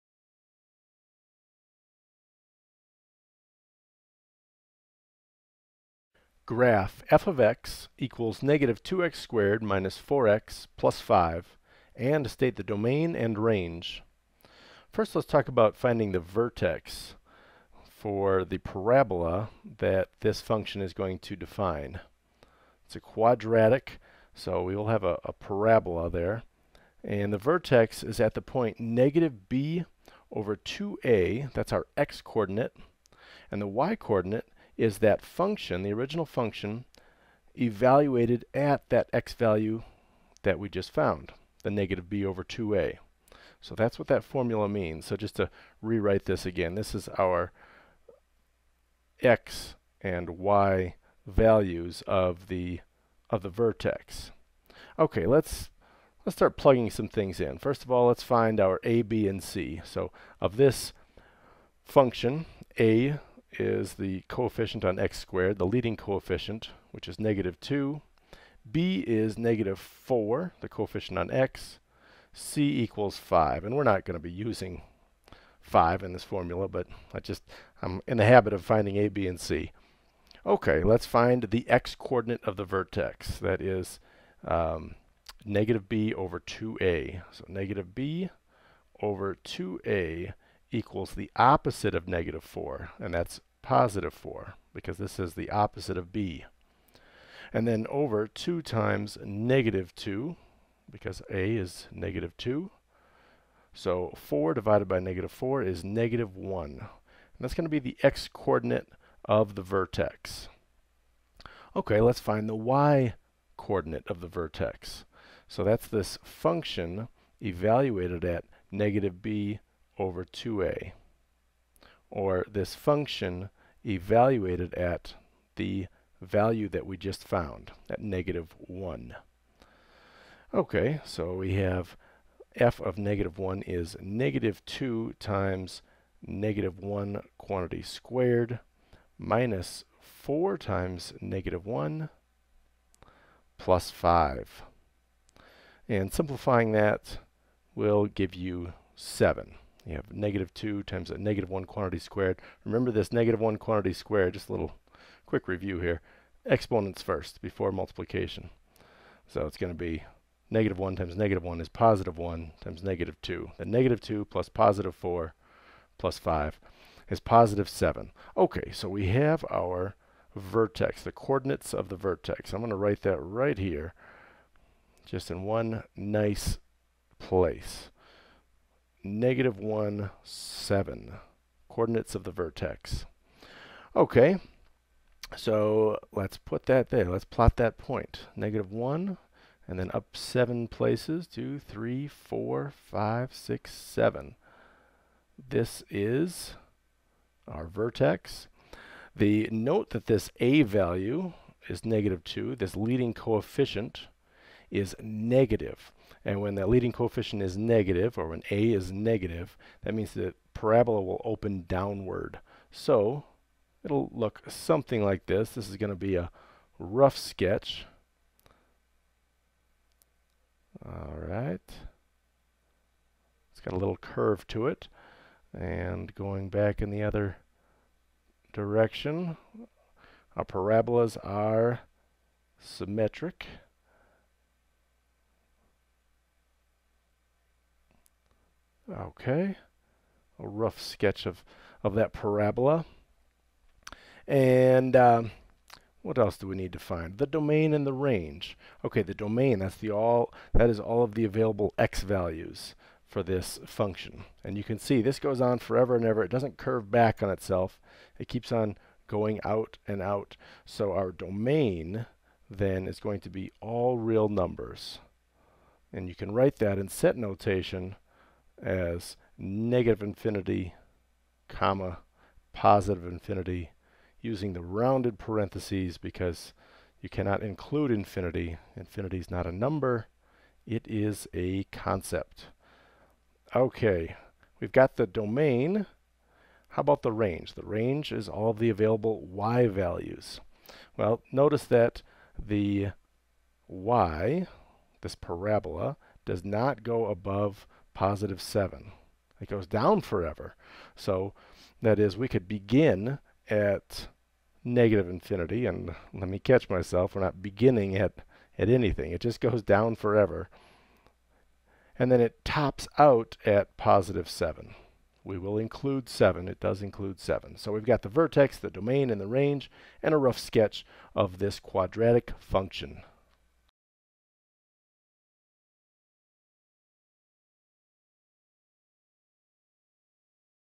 How to Graph General Quadratic Functions - College Algebra Tips
Learn How to Graph General Quadratic Functions in this College Algebra tutorial.  Watch and learn now!  Then take an online College Algebra course at StraighterLine for college credit.